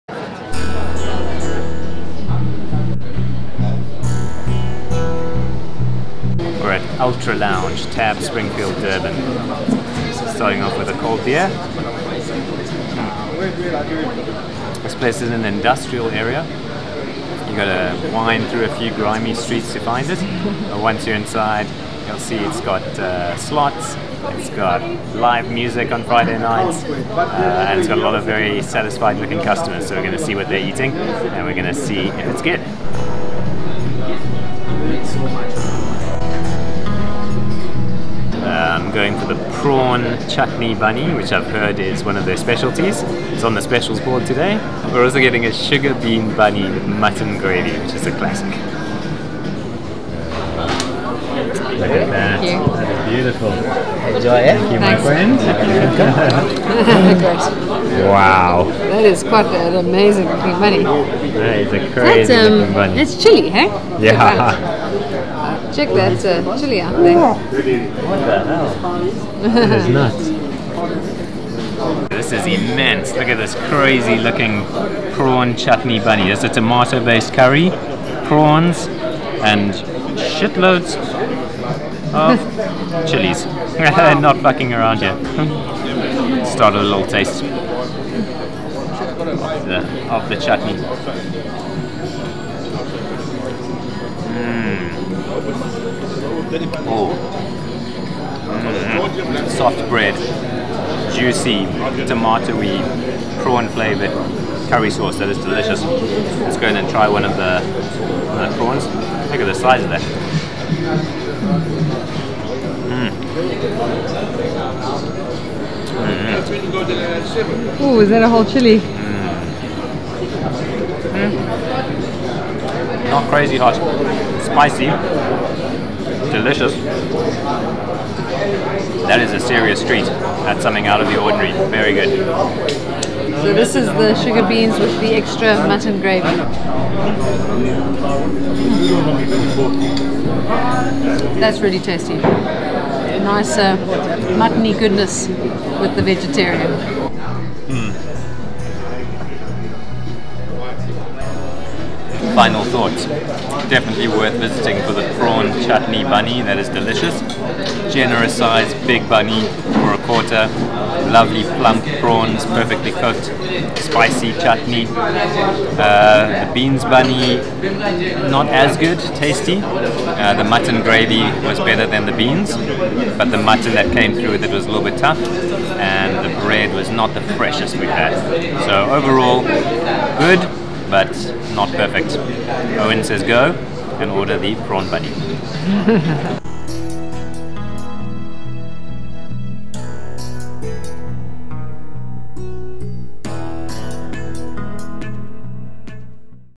Eating PRAWN chutney bunny chow at TSD
Tab Springfield Durban is a bustling horse racing tote with a popular restaurant and pub attached set in an industrial area squeezed between Inanda Road and the Umgeni River.

Best known for their famous prawn chutney bunny, TSD also offers less common fare, including such dishes as tripe with beans and Cornish chicken curry.

We found the prawn bunny (R130) to be an out of the ordinary experience with plenty of plump prawns and a rich tomatoey gravy. The much cheaper beans bunny with mutton gravy was not as good, but at R30, who can complain?

The atmosphere was lively, service was fast and friendly and beers were cold.